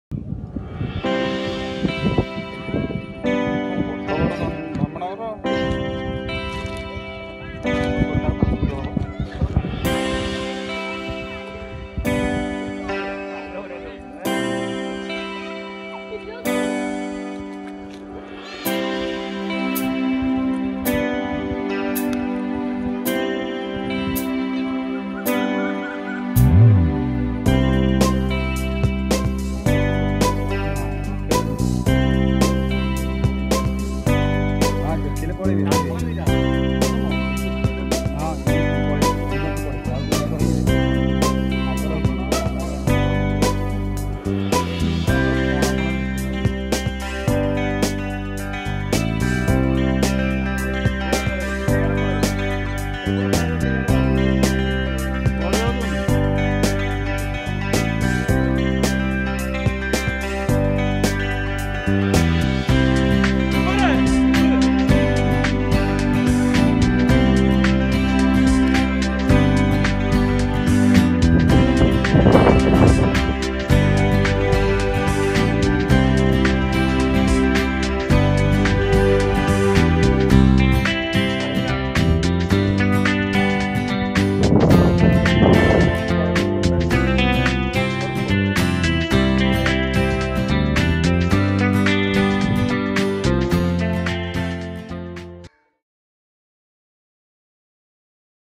WATCH: 2 Snakes playing in the pond
इंटरनेट पर एक दुर्लभ वीडियो वायरल हो रहा है जिसमें ओडिशा के खोरधा जिले के तालाब में 2 सांप (एक कोबरा और एक ओरिएंटल रैटस्नेक) खेलते नज़र आ रहे हैं।